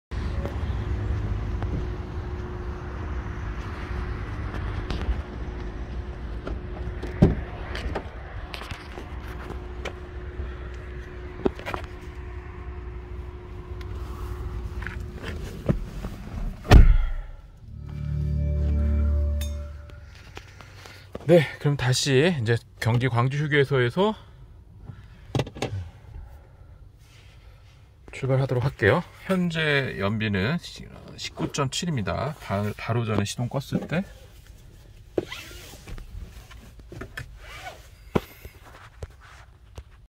[더카]아직도 철철 남아? XM3 이테크 계약자 필수 시청 시승기, 1.3 TCe 가솔린 장거리 연비 공개
자동차전문기자채널 더카입니다.

가장 빠르게 자동차 이야기를 보고 읽는 방법

XM3 신형 가솔린 TCe260 (제조일자 2022년 3월)

※더카의 리얼 체크 실차 테스트 드라이브입니다.